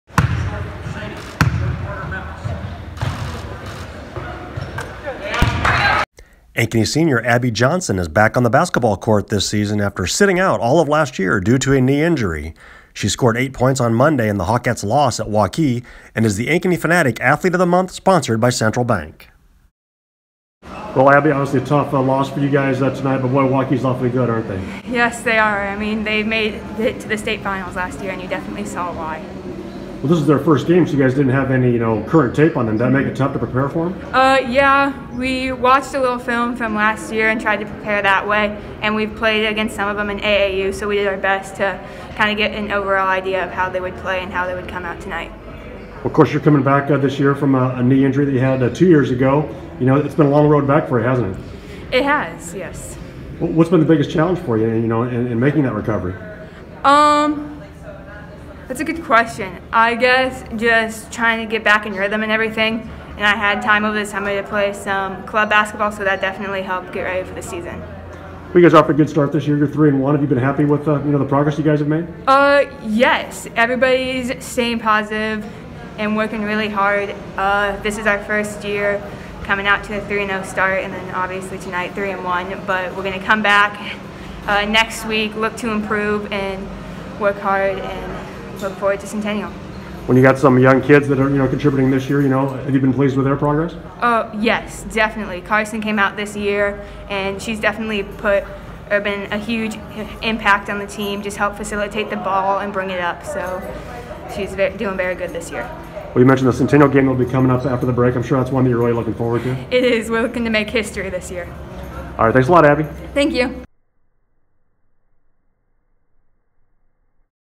Central Bank Athlete of the Month: Abby Johnson, Ankeny senior
The Northern State recruit is back on the basketball court this season after missing all of last year due to a knee injury.